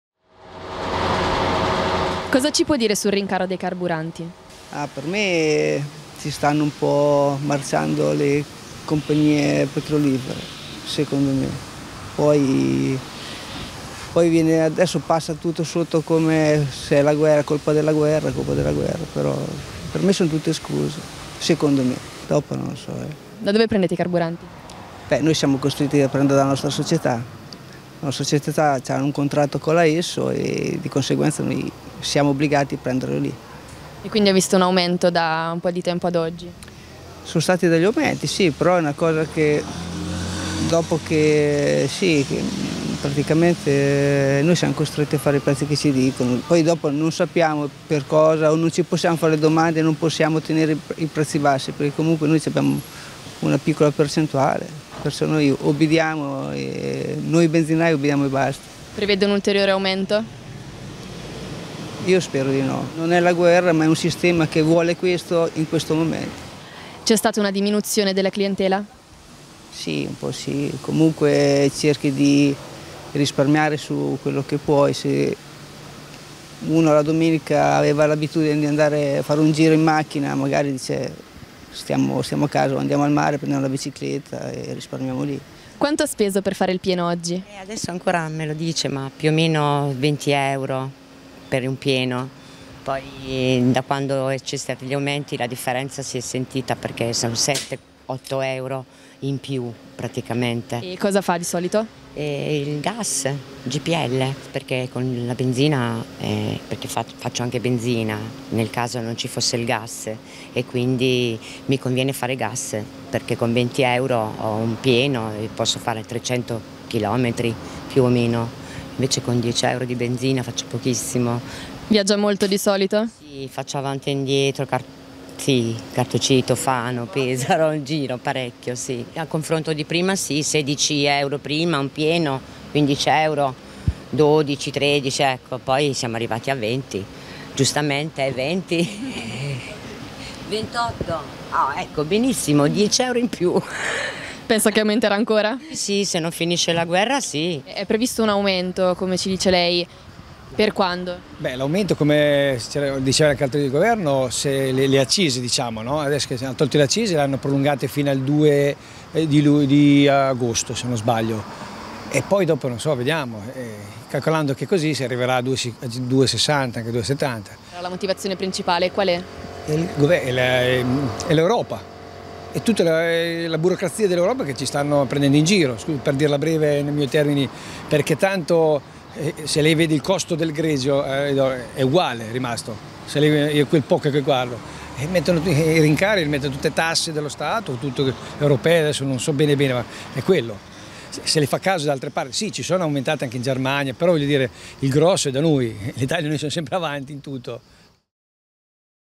Rincaro carburanti: benzina e diesel di nuovo sopra i due euro
L’aumento dei prezzi di benzina, diesel e GPL non si ferma. Nonostante il taglio delle accise, i carburanti tornano a superare i 2 euro al litro. Novità anche per il metano, non più tanto economico, che supera la quota di 2 euro al kg con un prezzo medio che va da 1,866 a 2,022.